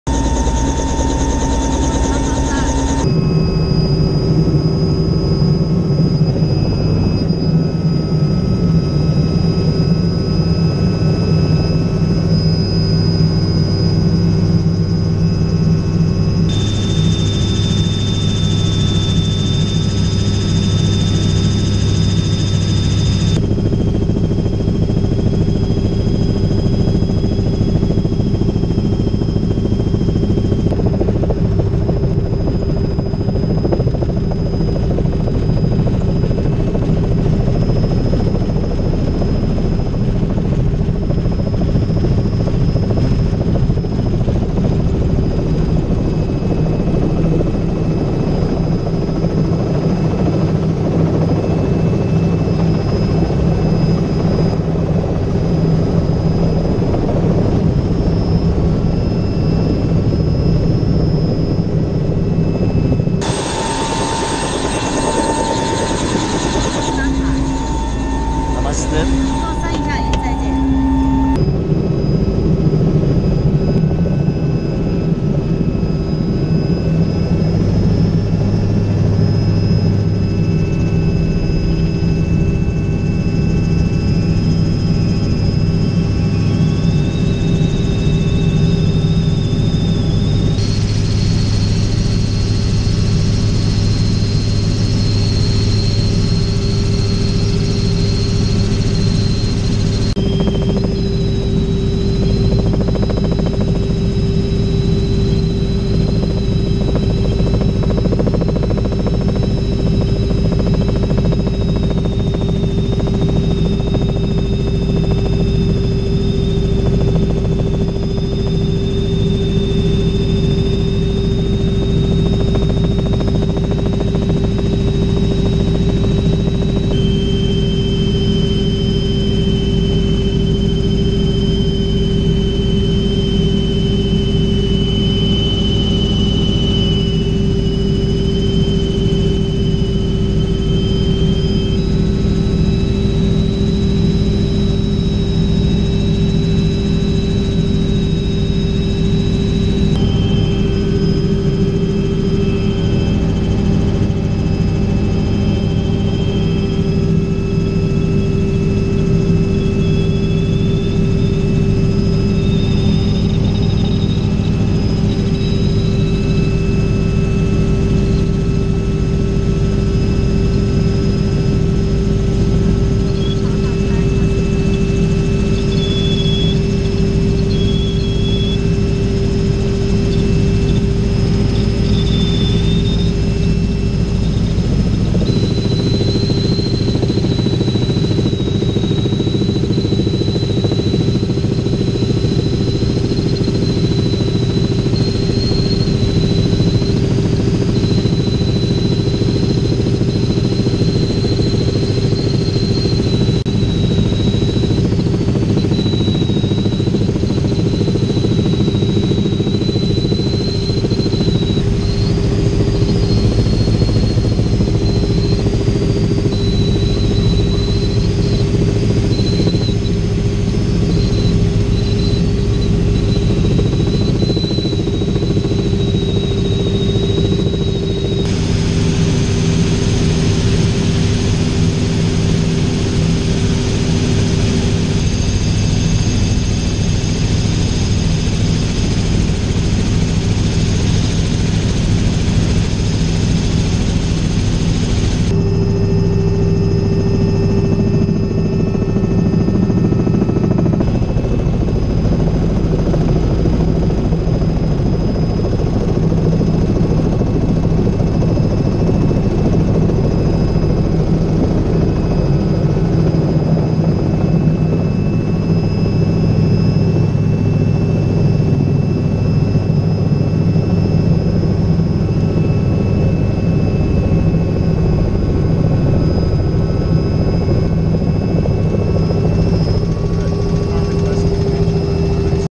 Helicopter Flight to Mardi Himal - Sightseing Annapurna Valley
Helicopter Flight to Mardi Himal
Sightseing Annapurna Valley, crossing over Machapuchare base camp and Annapurna Base Camp